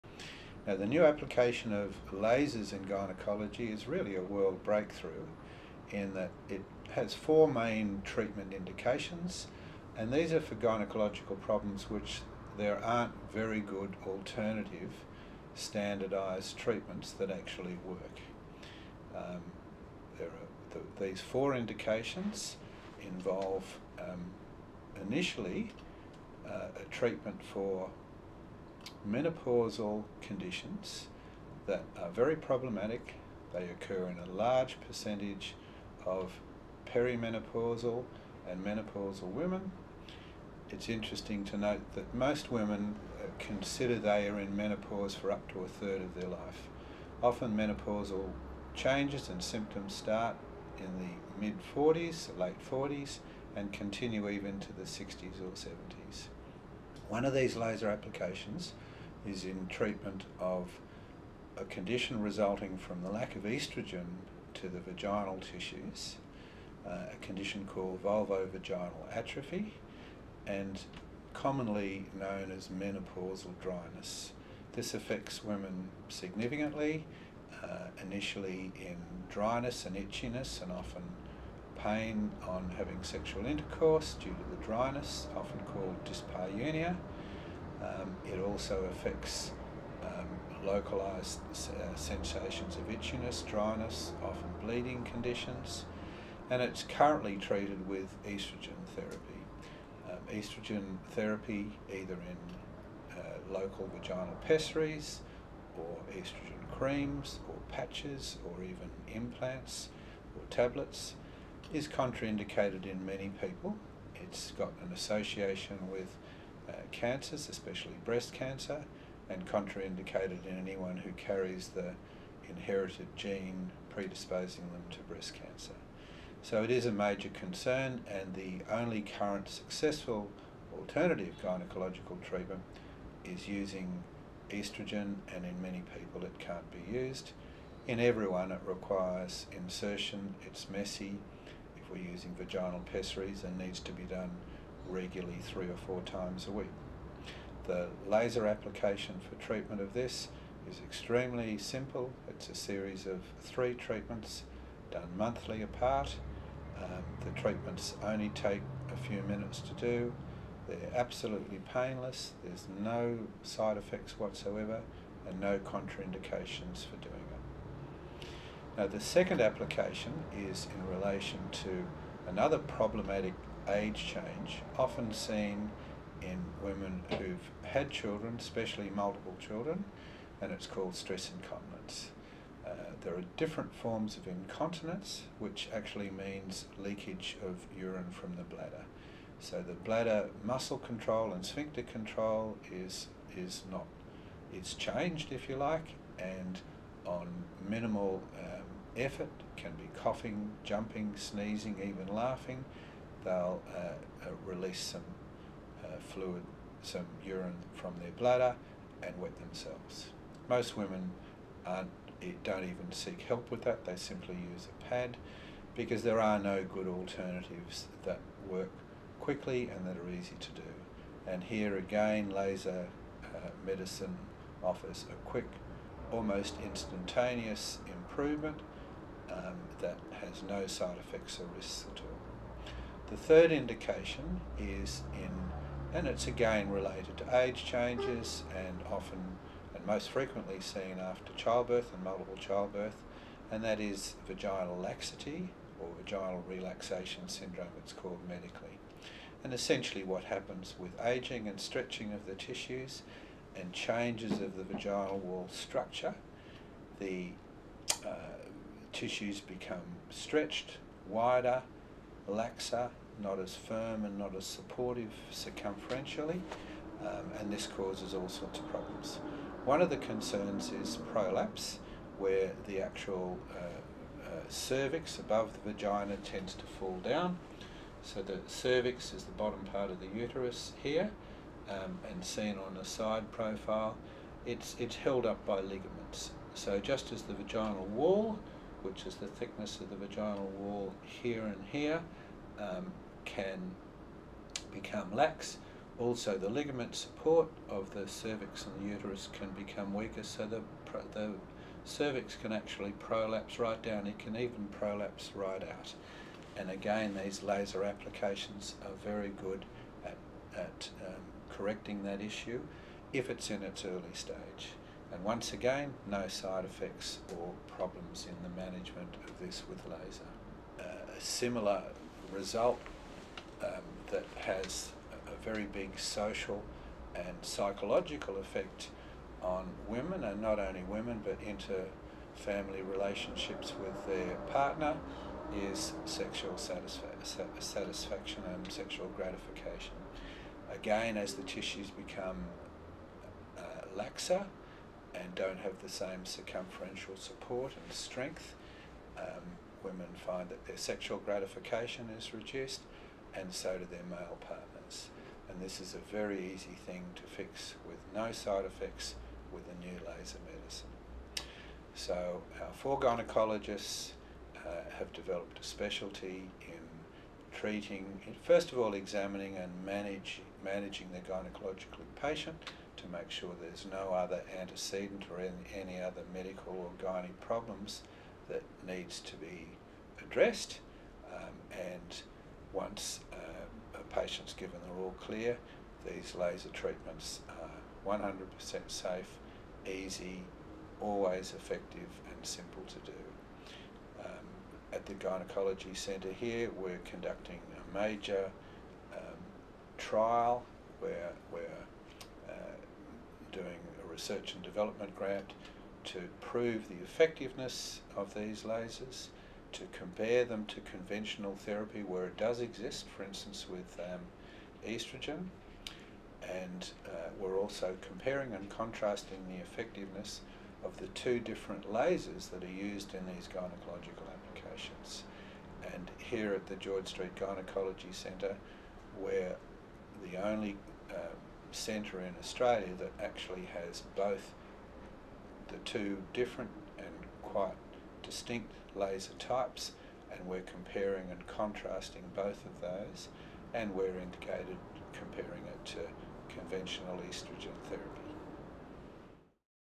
Laser Treatments For Gynaecology
Dr Garry Cussell, the principal doctor for Specialist Clinics of Australia gives a basic introduction on the laser treatments for various gynaecological issues.

For more information on laser treatments such as GynaeLase, MonaLisa Touch, IntimaLase, THermiVa and specialist gynaecological services to treat the issues discussed in this video visit our